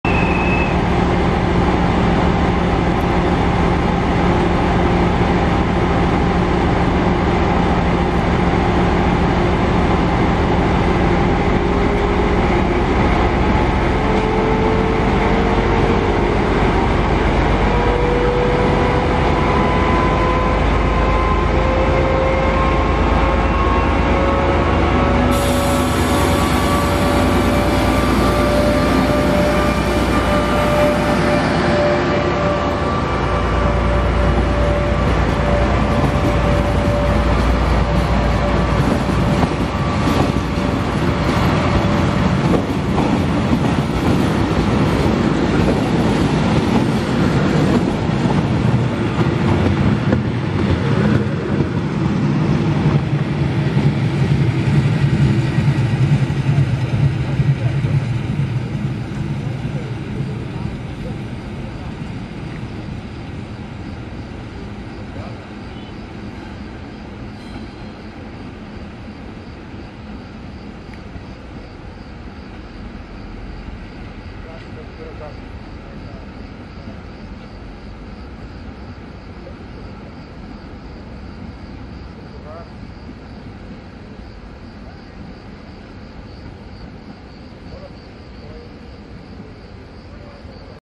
TRENO STORICO SULLA CEVA ORMEA ! Viaggio nell’arte dei Borghi dell’Alto Tanaro”
Ecco il atteso treno storico della Ceva Ormea di fondazione FS .. oggi composto da ben due locomotori diesel ( D445.1001 + D345.1118 ) in simmetria con alcune centoporte in composizione al treno . Il treno è in partenza da Ceva in direzione Torino Porta Nuova